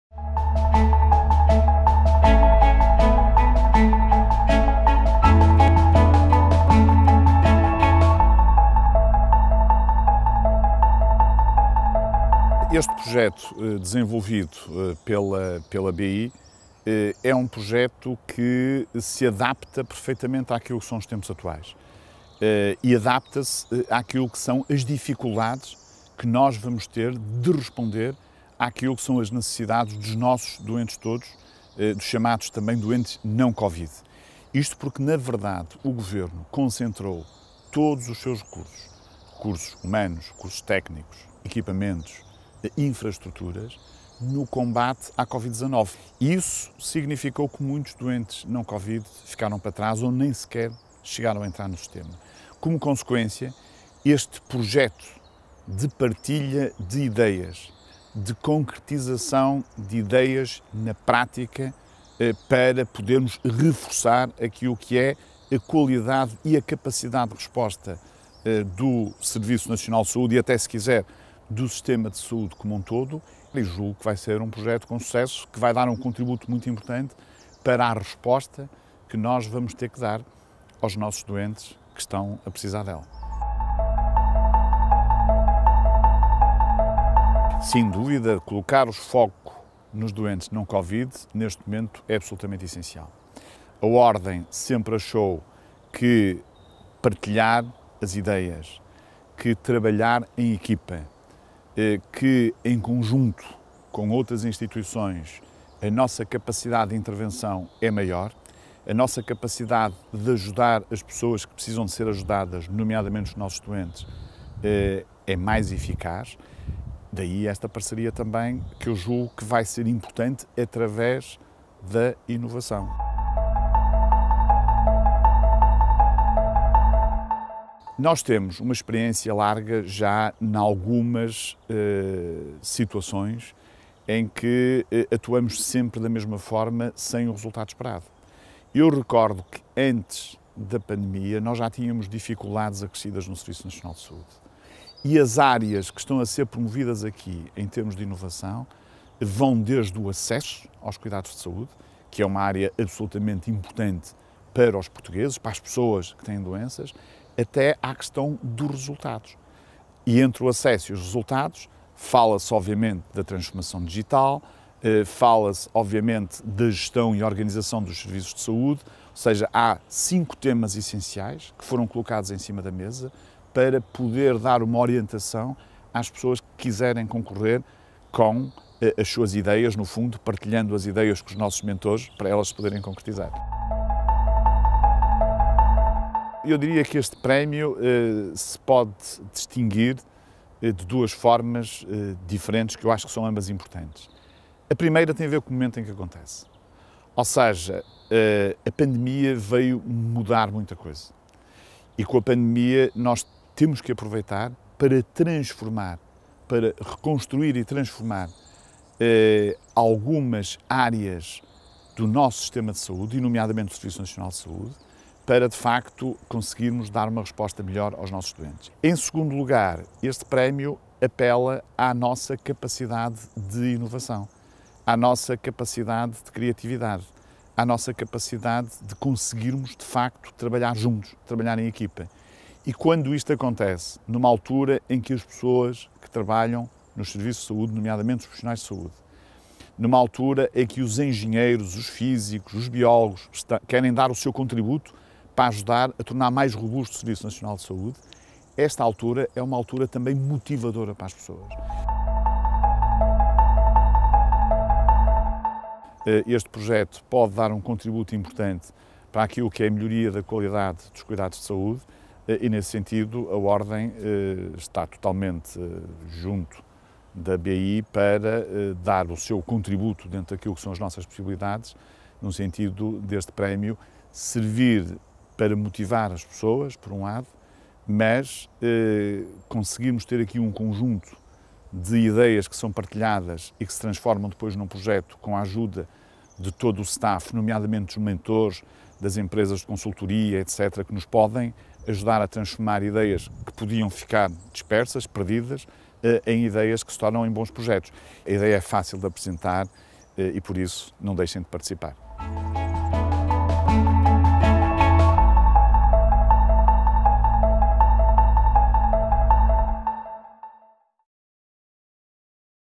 BI Award for Innovation in Healthcare: "Colocar o foco nos doentes não COVID-19 é essencial"
As candidaturas da competição BI Award for Innovation in Healthcare, uma iniciativa da Boehringer Ingelheim Portugal, com o apoio institucional da Ordem dos Médicos, terminam no dia 14 de abril, e já é conhecido o júri e o painel de mentores que vão ajudar os projetos a ganhar vida. Este é, de acordo com o bastonário da Ordem dos Médicos (OM), Dr. Miguel Guimarães, “projeto que se adapta, perfeitamente, àquilo que são os tempos atuais e às necessidades a que vamos ter de responder dos nossos doentes todos não COVID-19”.